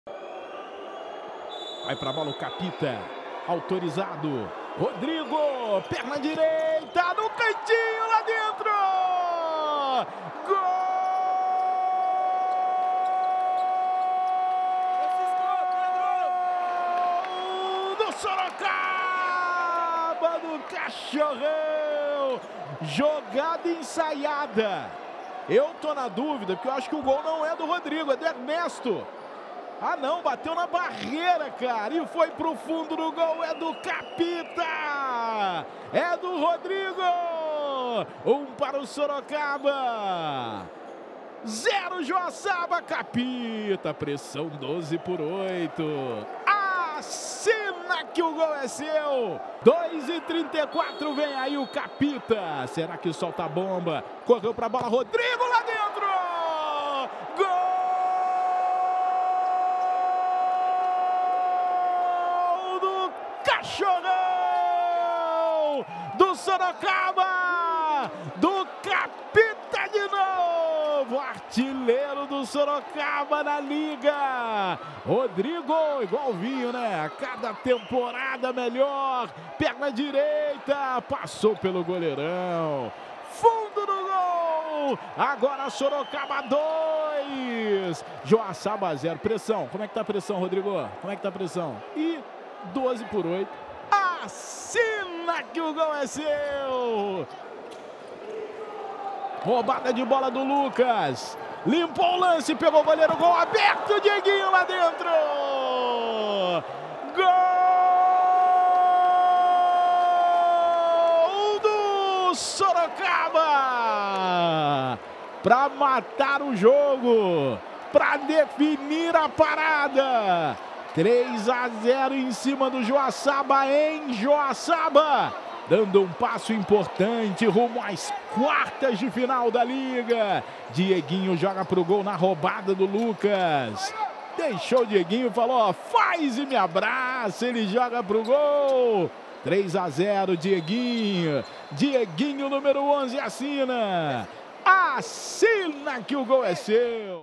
LNF MRJACK.BET 2023 - OITAVAS - IDA - GOLS - JOA 0 X 3 MAG
GOLS:
MAGNUS - RODRIGO (2) E DIEGUINHO
TRANSMISSÃO: SPORTV